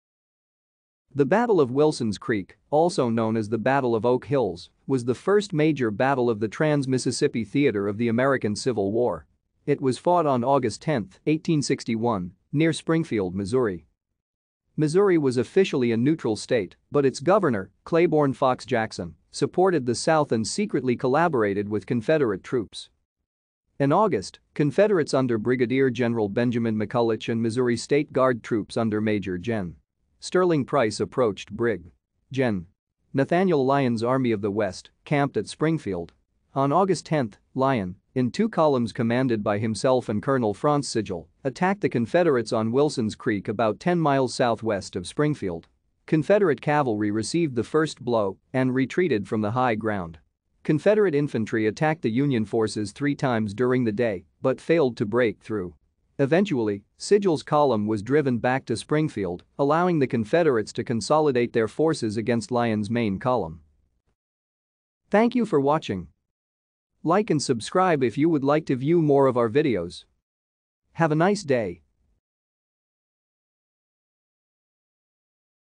Battle Of Wilson's Creek
The Battle of Wilson's Creek, also known as the Battle of Oak Hills, was the first major battle of the Trans-Mississippi Theater of the American Civil War. It was fought on August 10, 1861, near Springfield, Missouri.